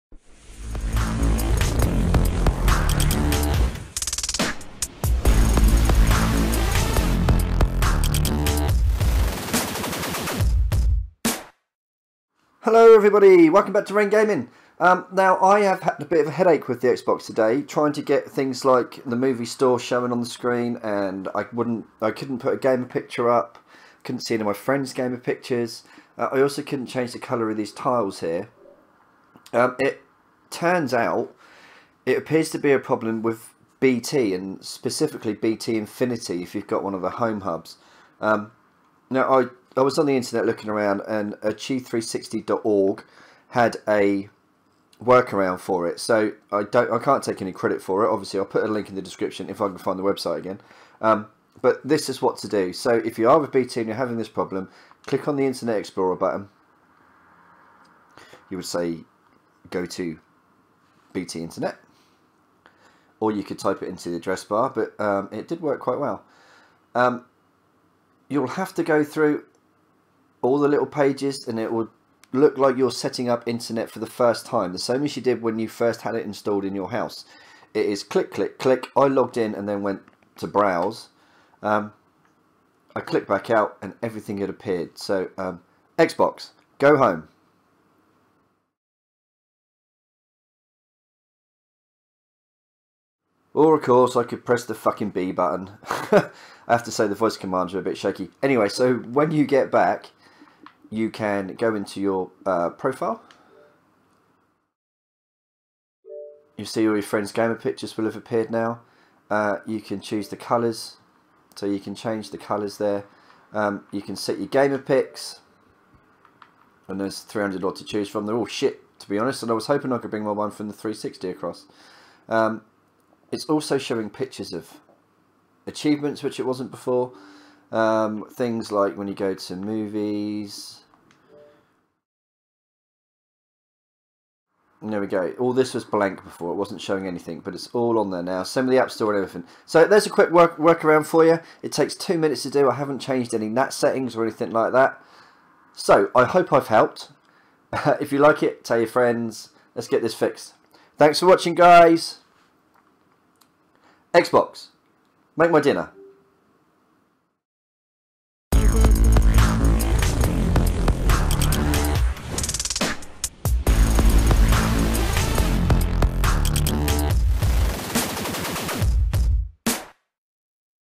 Xbox ONE Gamerpic and tile colour FIXED!
If you can't change your gamer pic, colour of the tiles, or see any movies then try this.